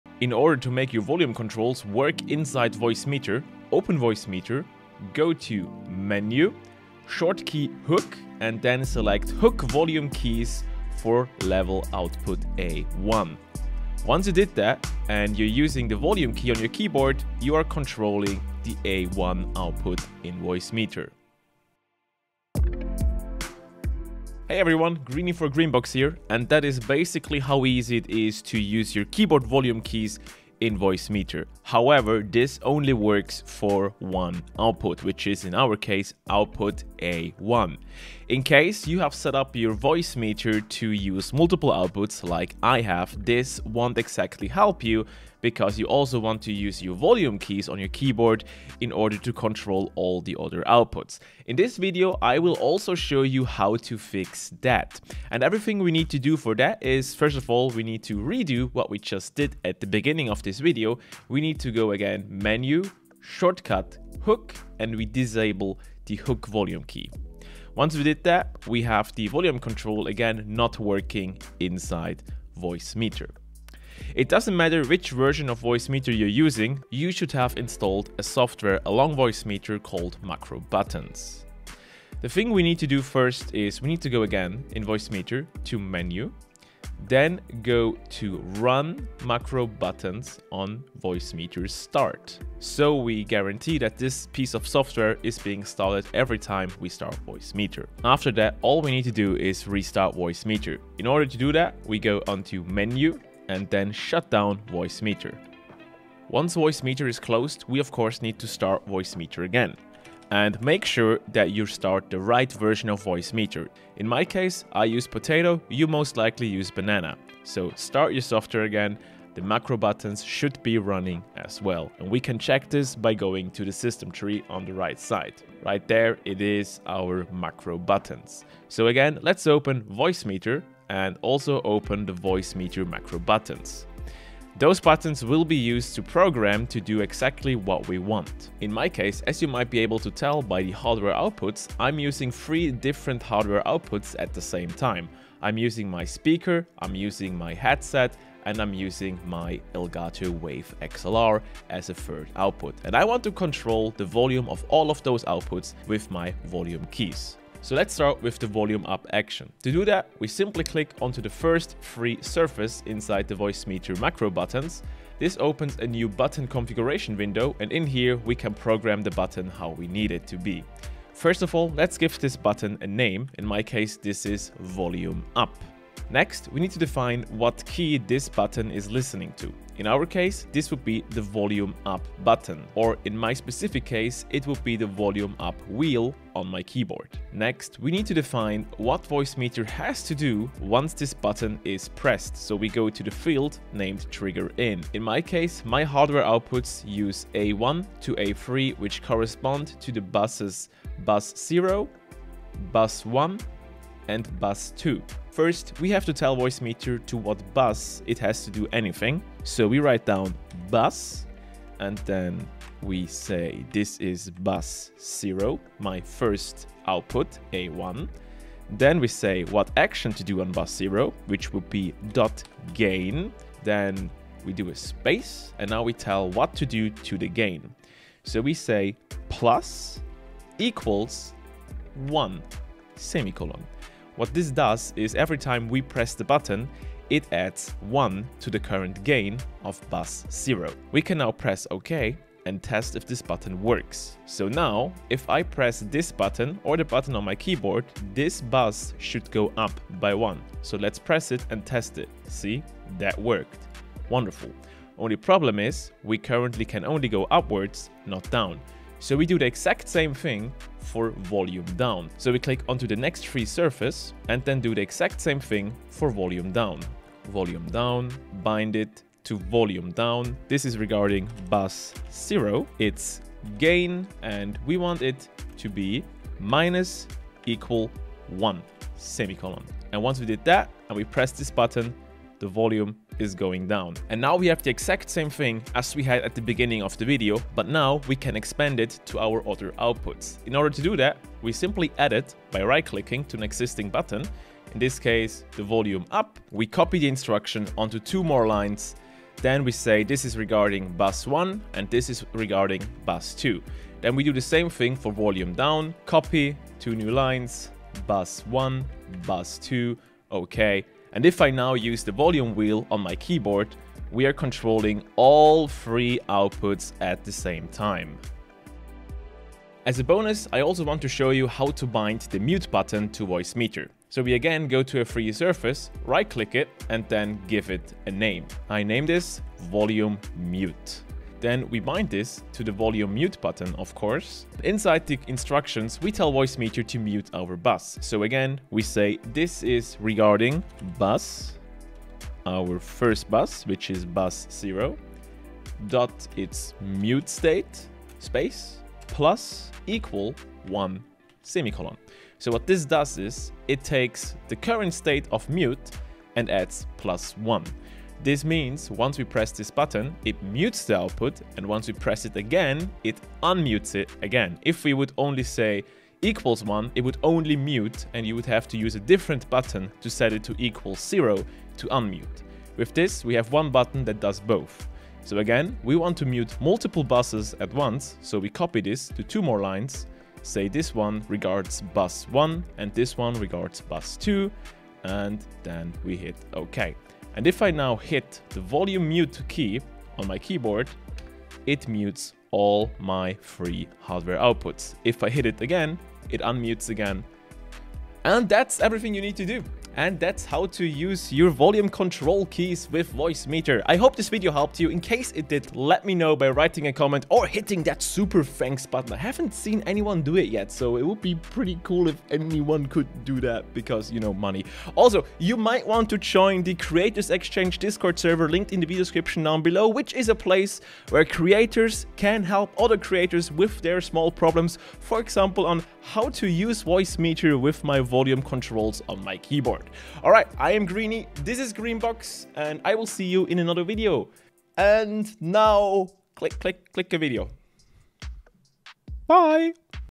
Voicemeeter Keyboard VOLUME CONTROLS | Macro Buttons
How to use keyboard volume control inside Voicemeeter (VOLUME UP, VOLUME DOWN, MUTE) using Voicemeeter macro buttons.

This guide is only using software that was installed with Voicemeeter. No additional Software is required.

Voicemeeter is a virtual audio mixing software which controls the audio of your computer. Unfortunately, for many live streamers, Voicemeeter does not listen to keyboard media and keyboard volume control by default. Using the Volume control of your keyboard for Voicemeeter can be as simple as activating one setting in the Voicemeeter options.

If you use a more complex Voicemeeter setup, using the volume control keys requires you to configure two small macro buttons.

This video will teach you both methods to enable Voicemeeter volume control buttons.

=== Macro Buttons Instructions  ===
Example for Bus 1 (A1)

bus[0].gain +=1;
bus[0].gain -=1;
bus[0].mute +=1;

00:00 Quick Tip
00:24 Introduction
00:56 Preparation for Macro buttons
02:13 Base setup explained
02:38 Creating Volume UP Control
04:19 Creating Volume DOWN Control
04:45 Expanding volume controls to more outputs
05:08 Creating MUTE control (Bonus Tip)
06:38 Conclusion and Outro